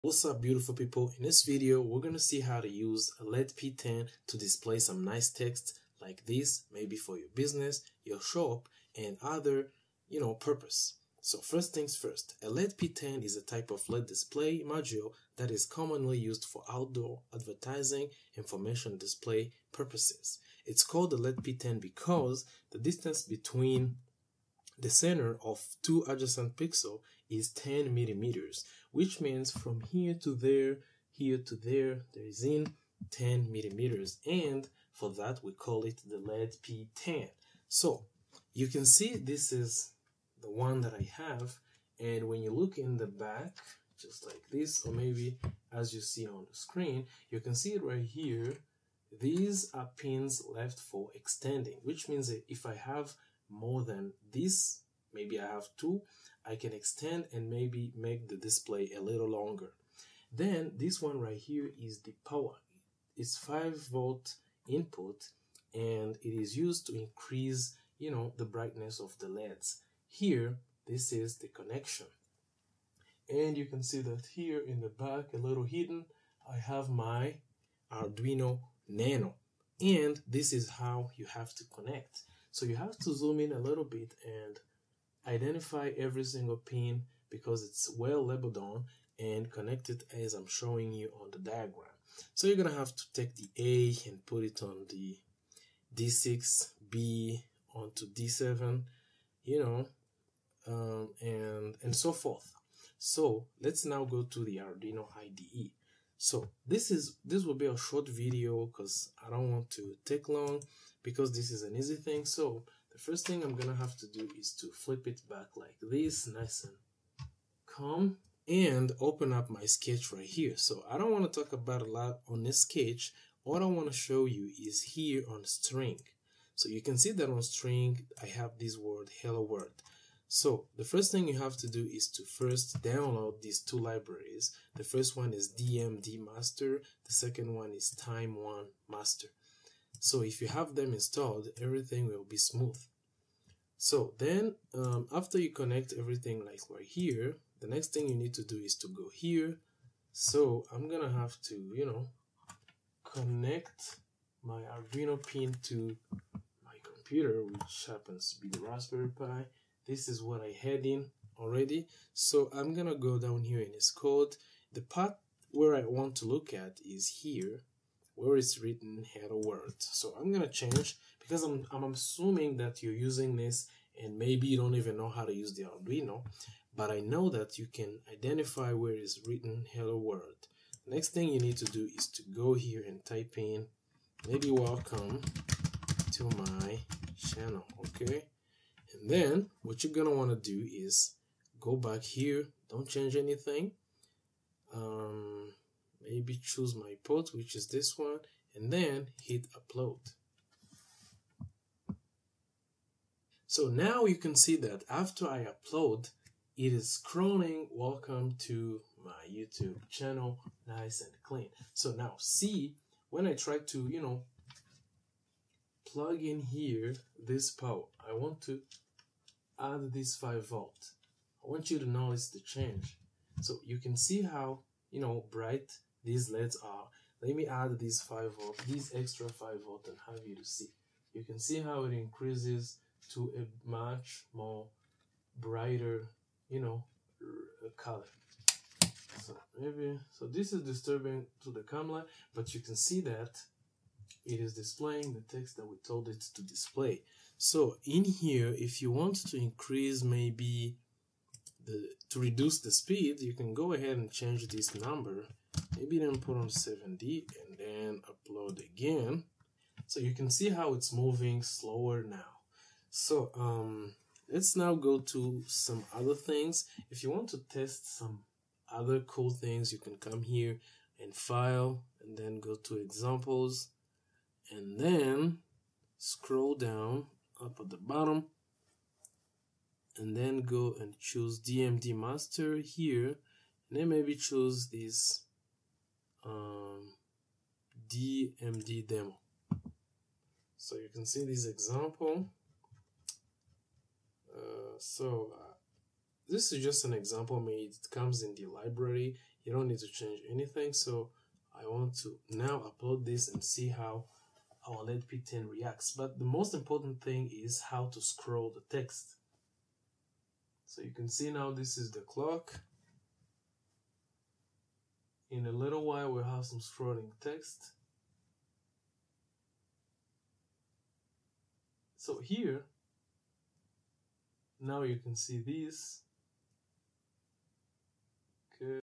how to make advertising display panel with led p10 and Arduino
In this video, we view how to use the led p10 to make scrolling text panel.